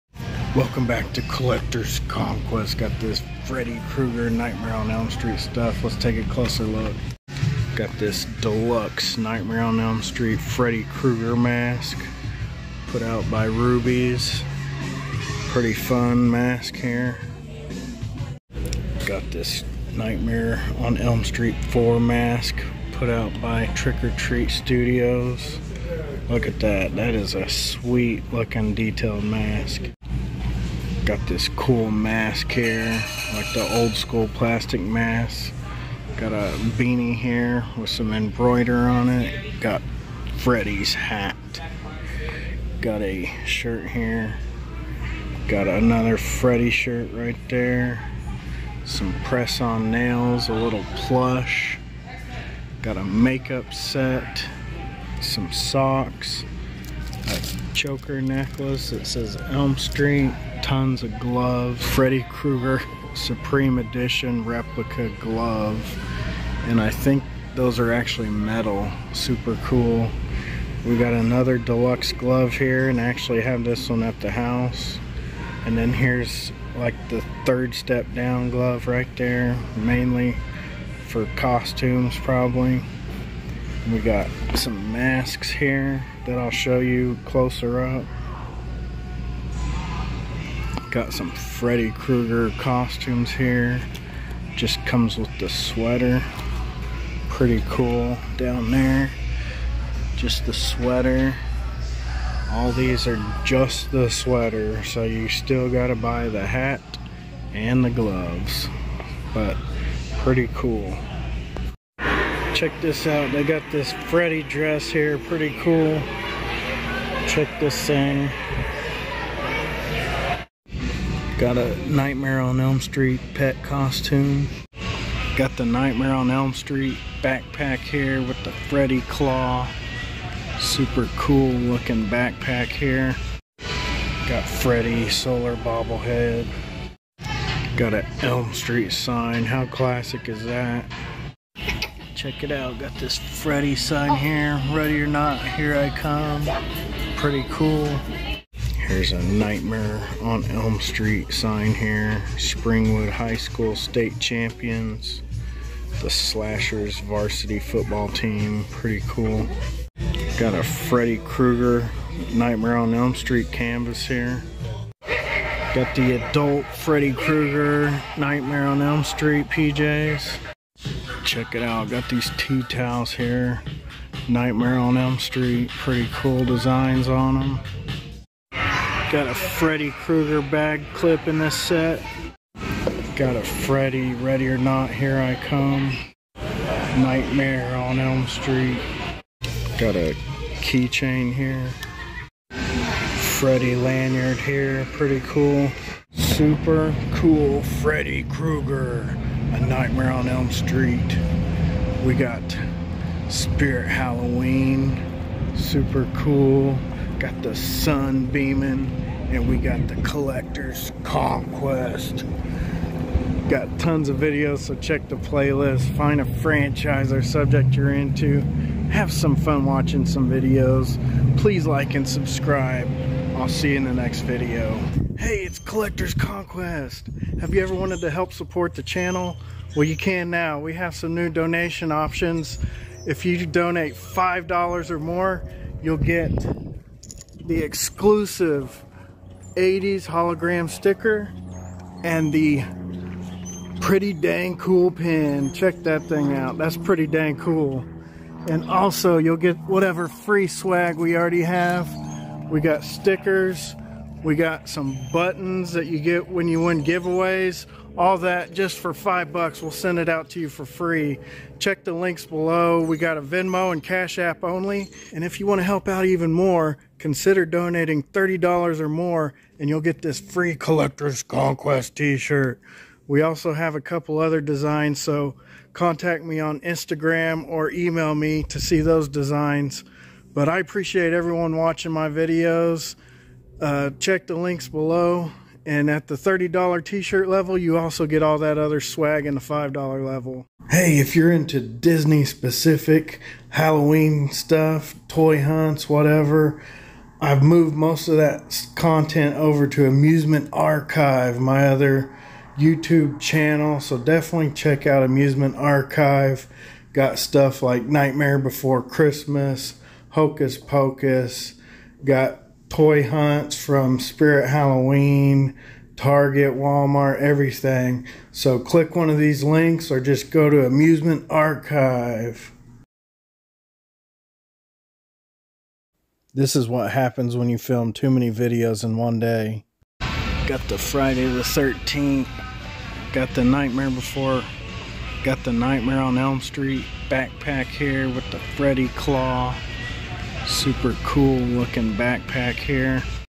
Freddy Krueger A Nightmare on Elm Street Spirit Halloween Full Guide 2023
Brand new A Nightmare on Elm Street movie items at Spirit Halloween!

All designs subject to change. *Please allow a few weeks for t-shirt delivery.

Mailing Address:
Collector's Conquest
941 West I35 Frontage Rd